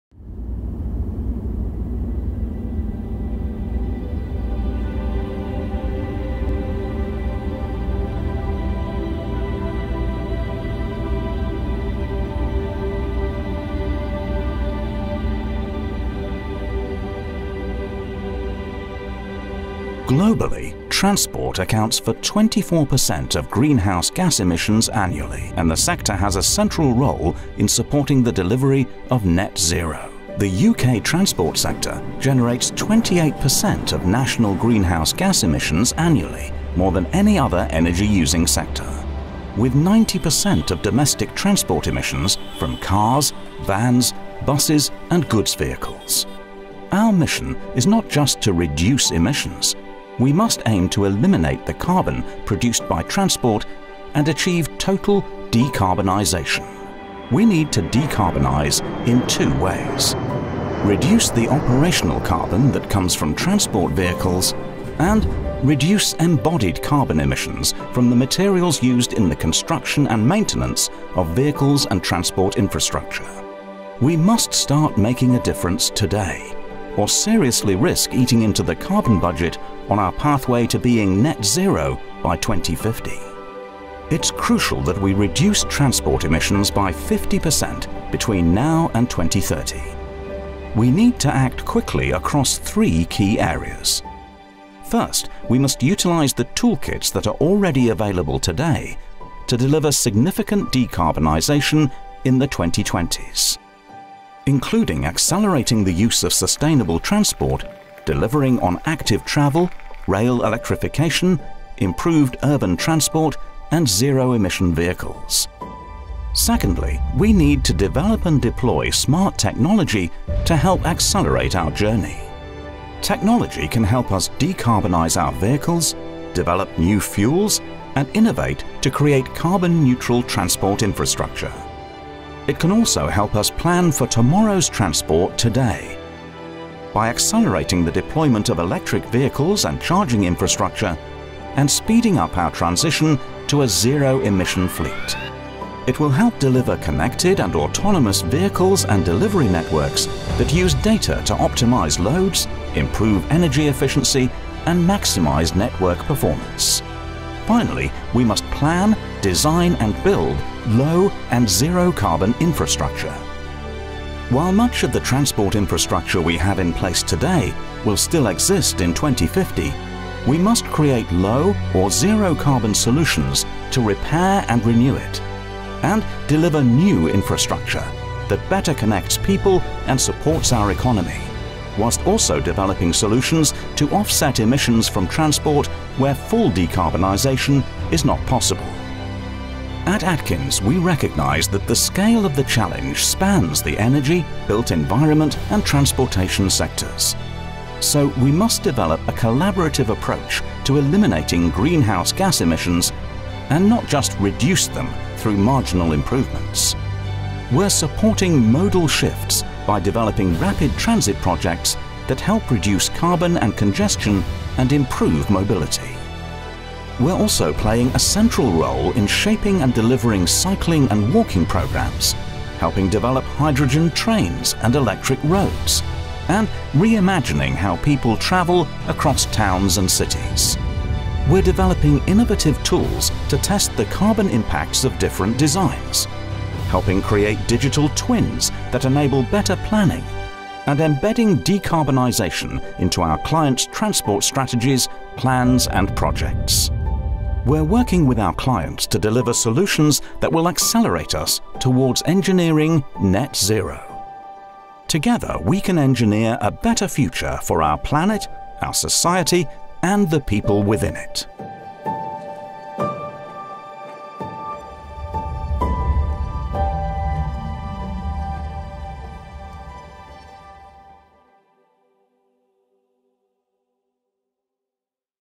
Engineering Net Zero in Transport
Find out how we’re Engineering Net Zero in transport by utilising existing toolkits, developing smart technology and designing low and zero-carbon infrastructure to create a better future for our planet, our society, and the people within it.